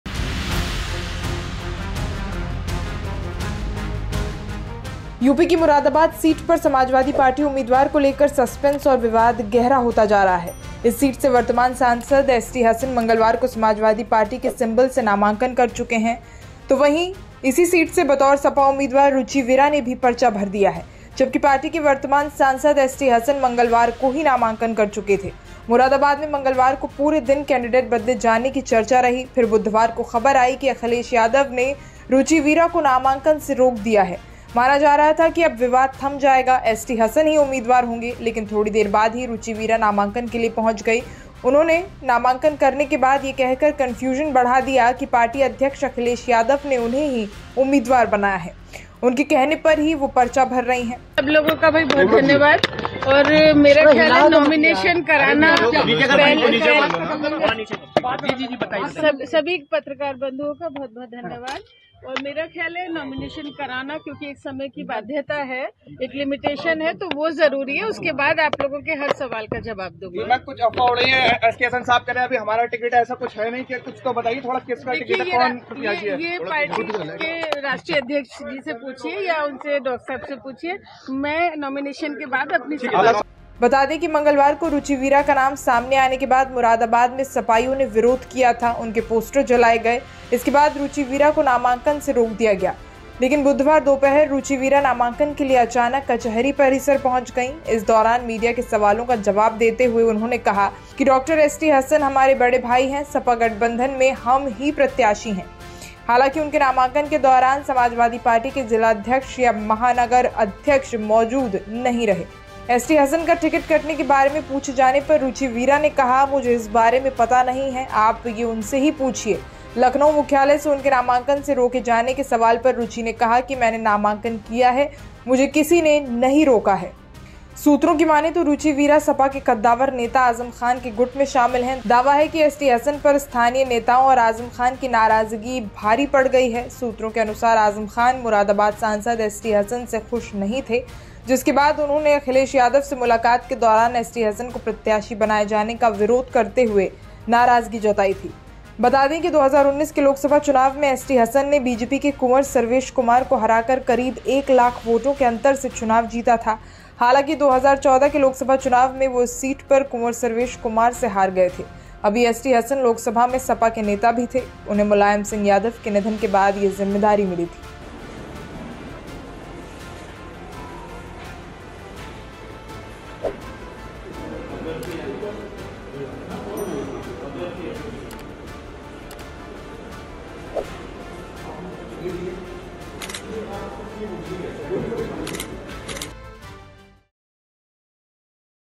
Lok Sabha Election: Moradabad से ST Hasan के बाद Ruchi Veera ने भरा नामांकन, SP Candidate कौन?
मुरादाबाद सीट पर समाजवादी पार्टी उम्मीदवार को लेकर सस्पेंस और विवाद गहरा गया है। इस सीट से वर्तमान सांसद एसटी हसन मंगलवार को समाजवादी पार्टी के सिंबल से नामांकन कर चुके हैं। पार्टी उम्मीदवार को लेकर कंफ्यूजन बढ़ गया है। बुधवार को इस सीट से बतौर सपा उम्मीदवार रुचि वीरा ने भी पर्चा भर दिया। जबकि पार्टी के वर्तमान सांसद एसटी हसन मंगलवार को नामांकन कर चुके थे। मुरादाबाद में मंगलवार को पूरे दिन कैंडिडेट बदले जाने की चर्चा रही। फिर बुधवार को खबर आई कि अखिलेश यादव ने रुचि वीरा को नामांकन से रोक दिया है।